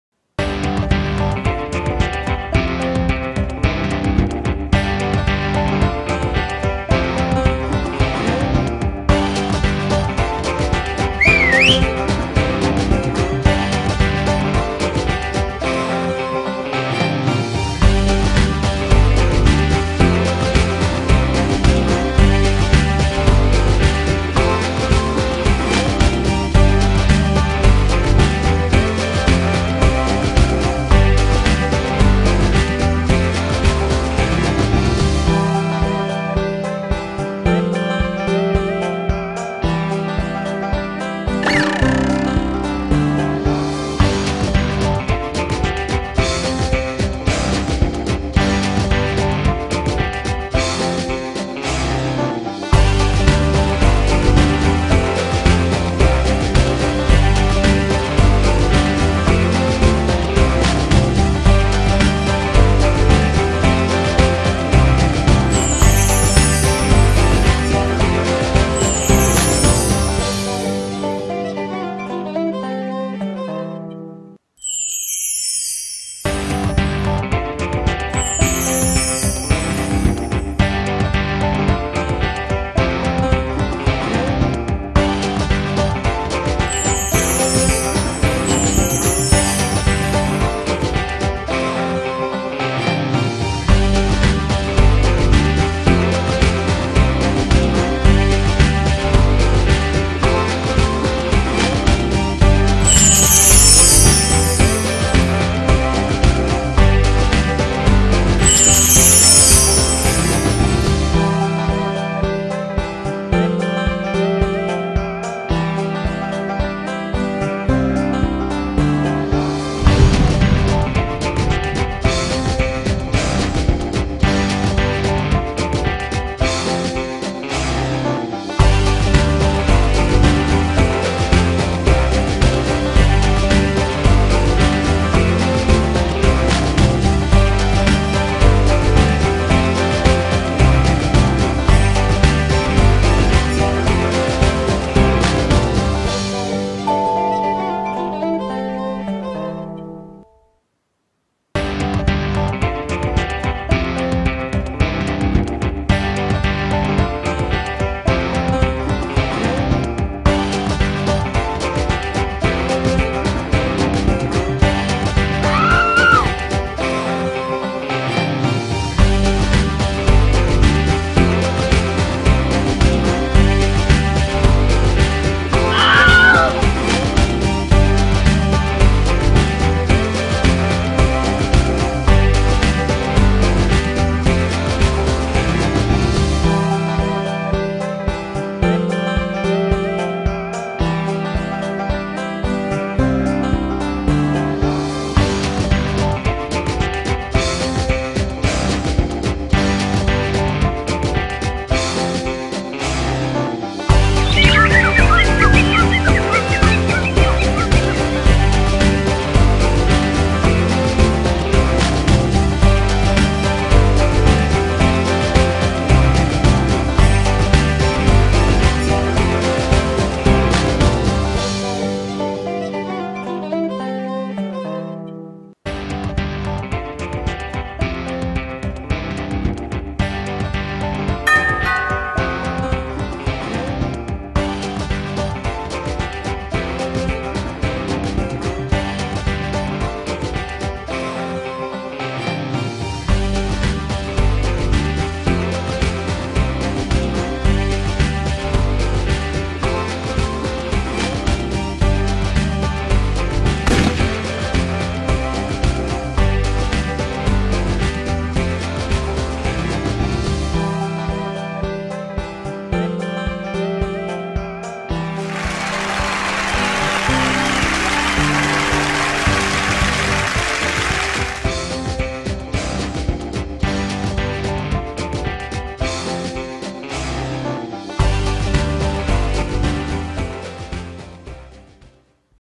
How to make a waterproof Caricature marker from a Crayola
Here are some of the supplies on Amazon.

Crayola marker maker: body and interior and tip of marker

Higgins waterproof ink

Good luck!